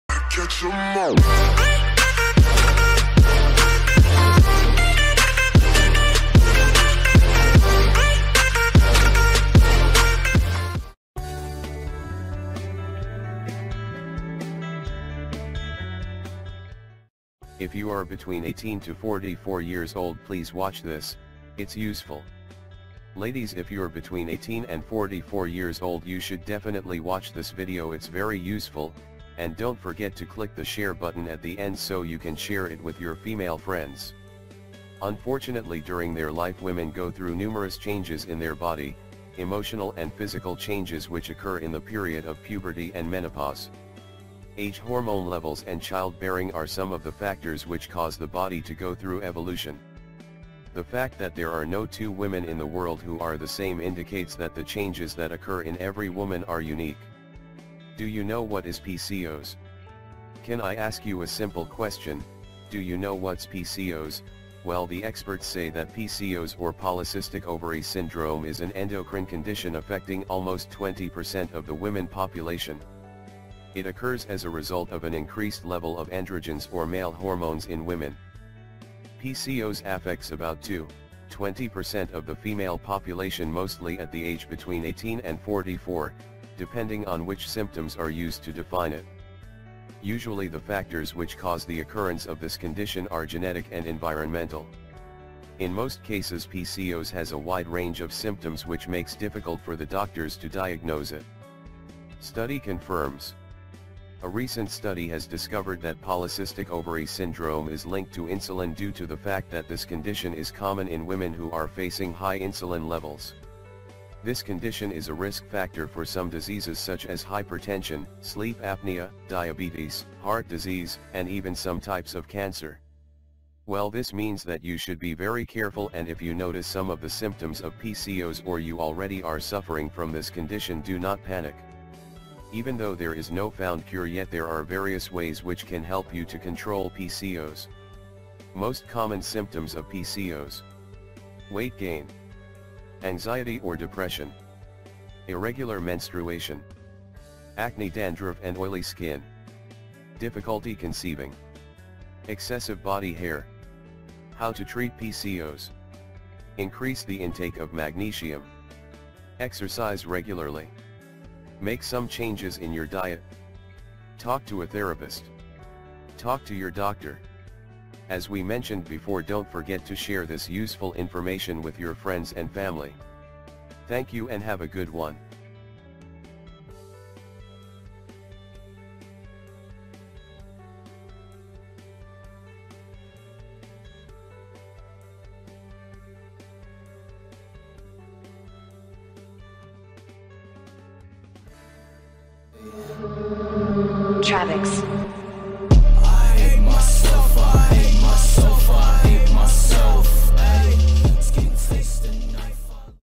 If You Are Between 18 44 Years Old Please Watch This It's Useful
Polycystic ovary syndrome PCOS is a problem in which a woman’s hormones are out of balance.
It can cause problems with your periods and make it difficult to get pregnant.

Signs and symptoms of PCOS include irregular or no menstrual periods, heavy periods, excess body and facial hair,
acne, pelvic pain, difficulty getting pregnant, and patches of thick, darker, velvety skin.
Associated conditions include type 2 diabetes, obesity, obstructive sleep apnea, heart disease, mood disorders, and endometrial cancer.

The exact cause of PCOS is unknown, but there could be a number of factors which contribute to the onset of this condition,
from genetic to environmental and the symptoms can be wide-ranged and vary from one individual to another.
If you’re suffering from PCOS or you suspect you might be dealing with this condition
first you need to consult with your doctor and determine for sure.